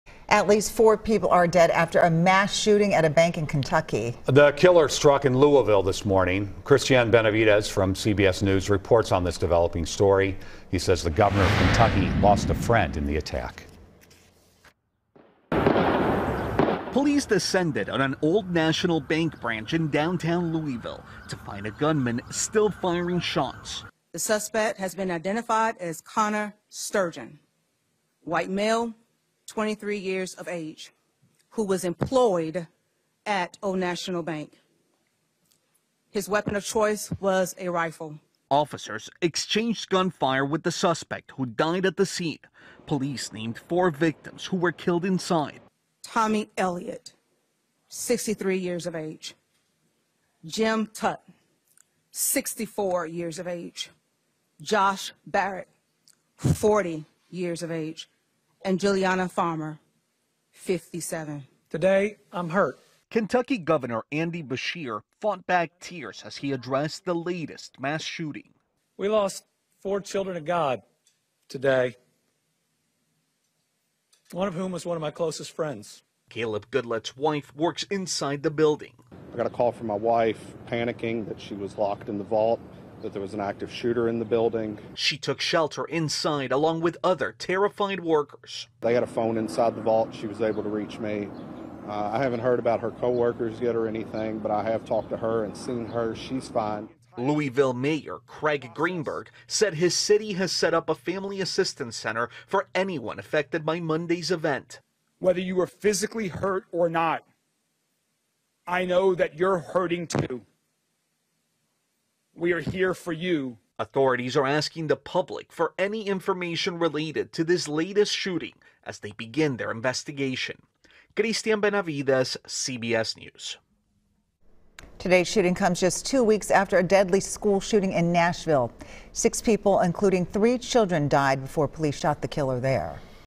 Kentucky’s governor says he lost friends in Louisville shooting
Police identified the four slain victims as Joshua Barrick, 40; Thomas Elliot, 63; James Tutt, 64; and Juliana Farmer, whose age has yet to be determined.